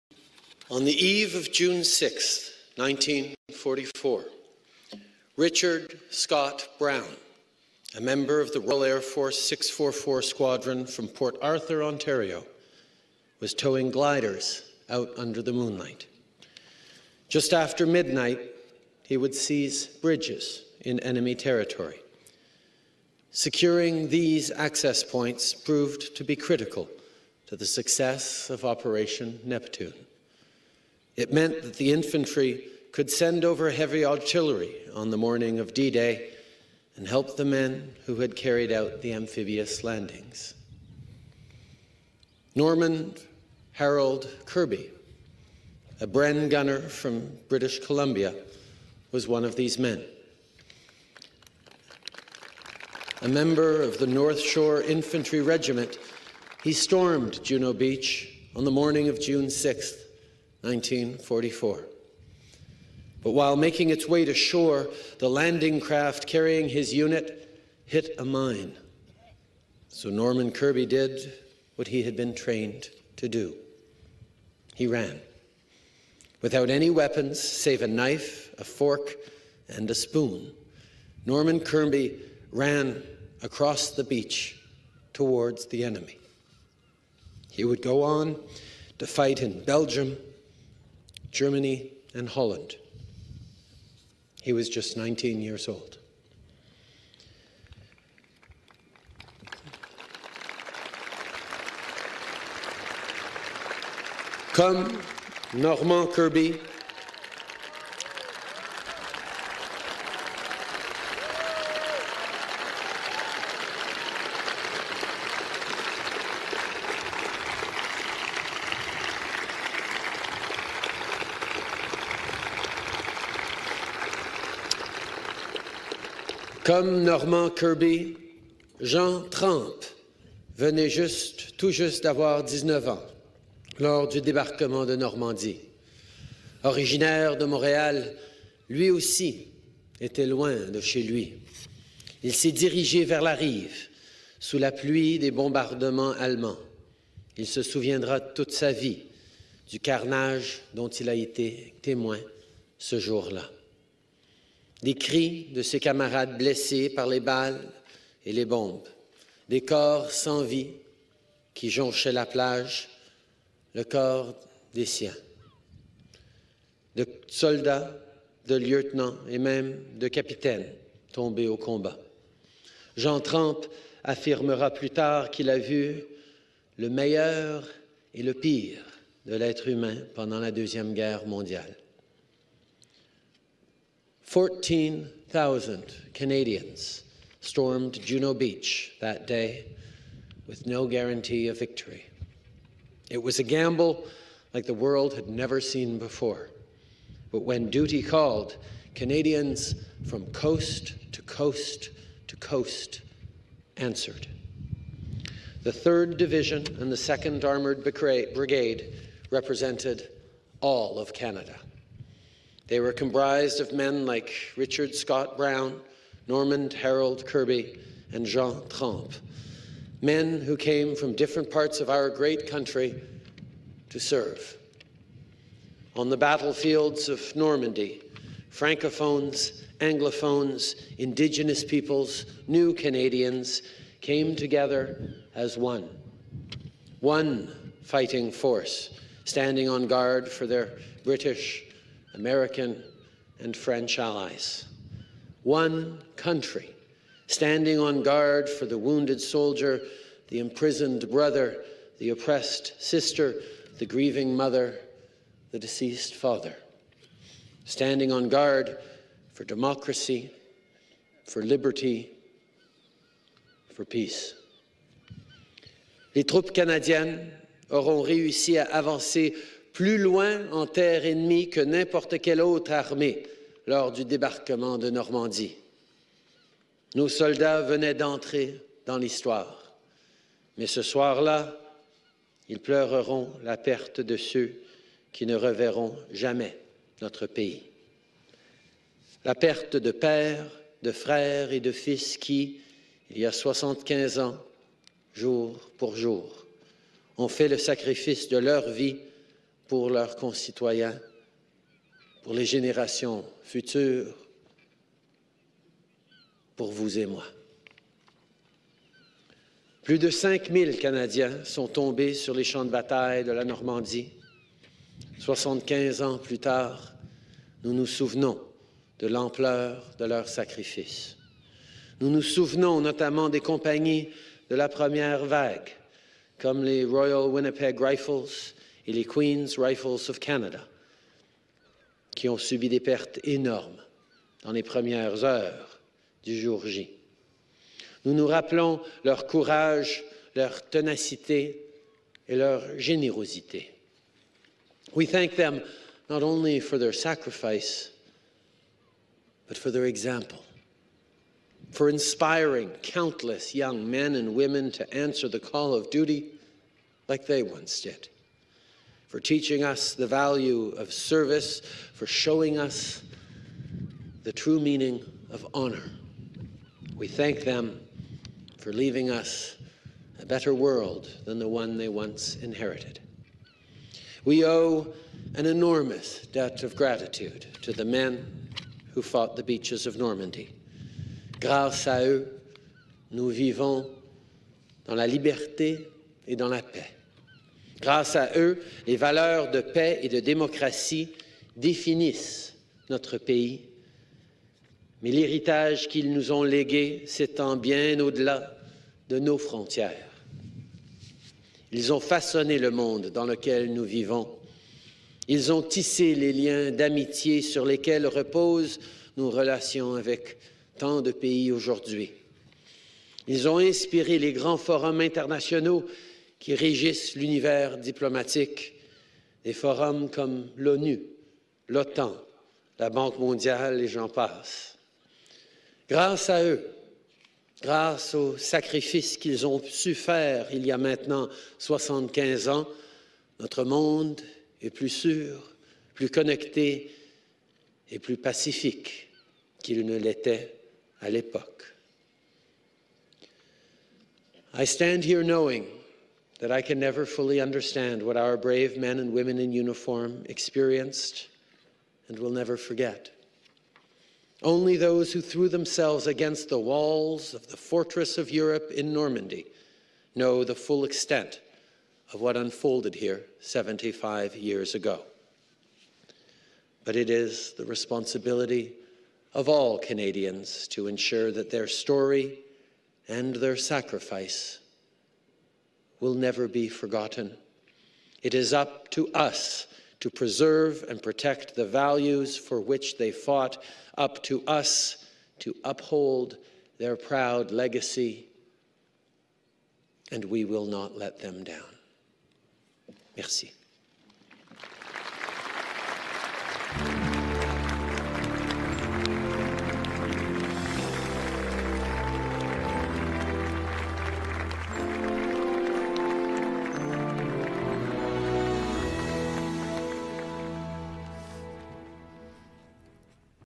Remarks on the 75th anniversary of D-Day and the Battle of Normandy
Prime Minister Justin Trudeau delivers remarks on the 75th anniversary of D-Day and the Battle of Normandy at Juno Beach in France.